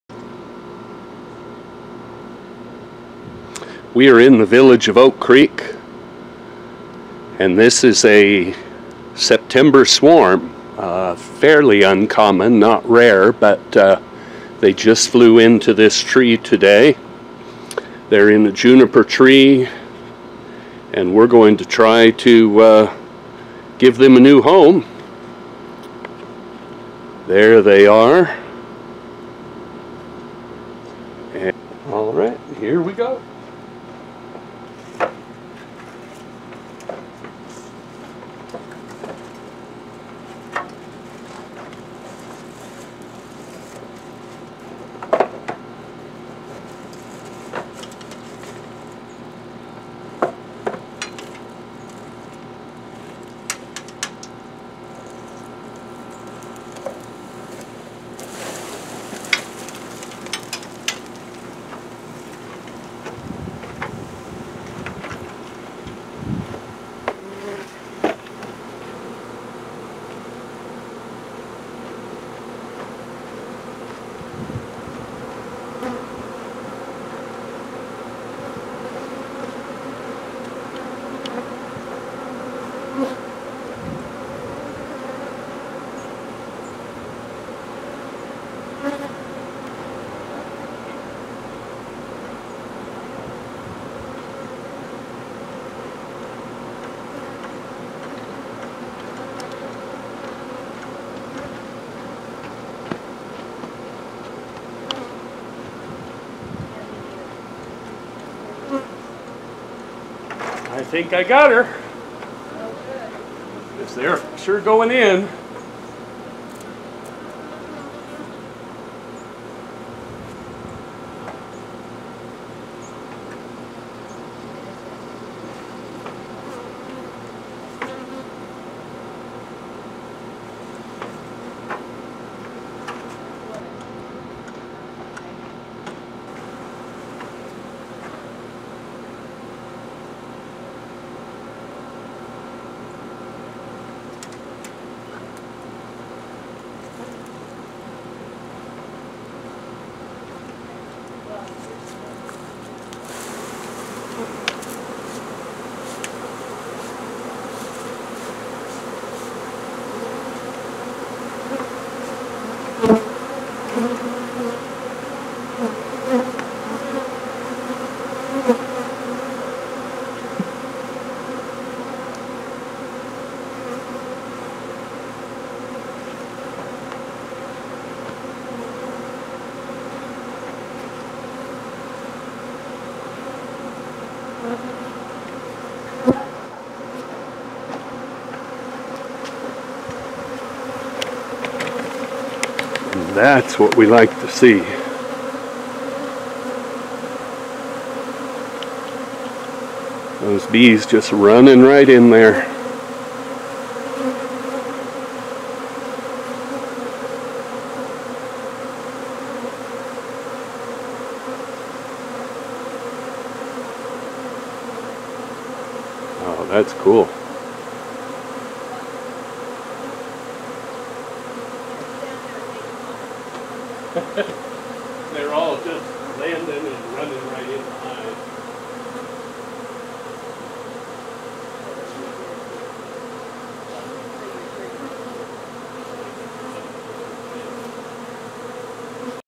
Bassous Swarm VOC
A contractor friend called that this swarm flew right over him and landed in a juniper tree.  This is the Village of Oak Creek, Sedona, Arizona.  September 9th, 2016.
Love free bees!